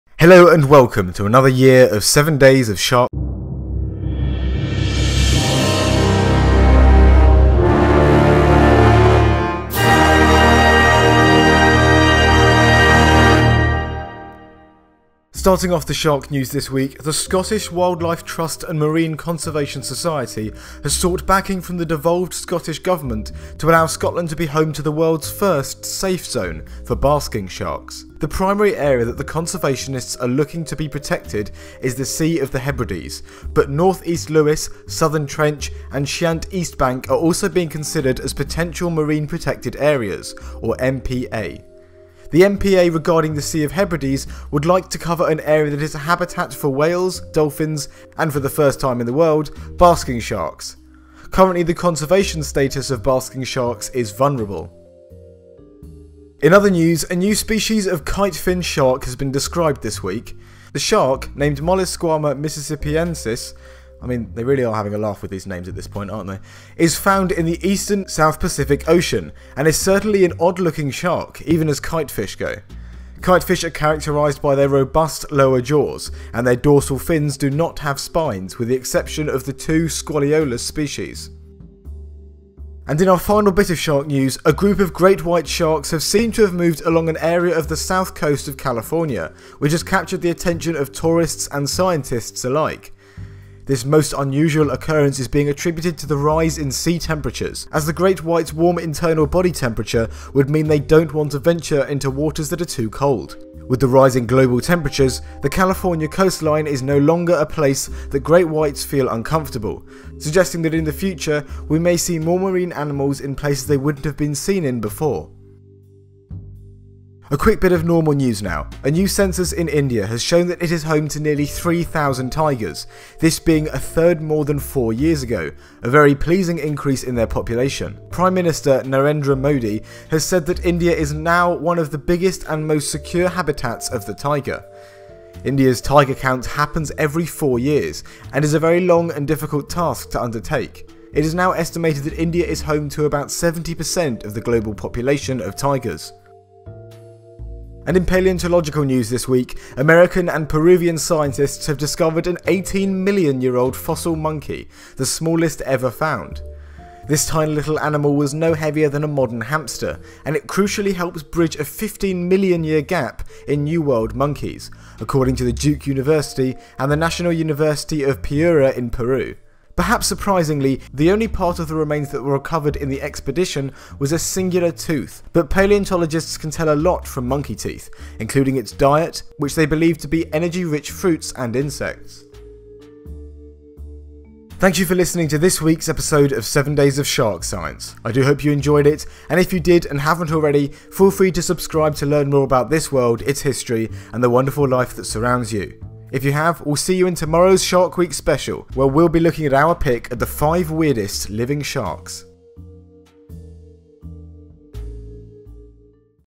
Great Whites in California & Basking Shark 'Safe Zone' - 7 Days of Shark Science
It's that time of year again, and this year, we're combining Seven Days of Science and Seven Days of Shark Science. Awesome! Not as awesome as digestives though. Apparently it's a British thing, which is ridiculously tragic because that means the rest of the world don't get to taste that chocolatey, biscuity wonder. Also sharks are cool. Did you know rays are sharks? Awesome huh?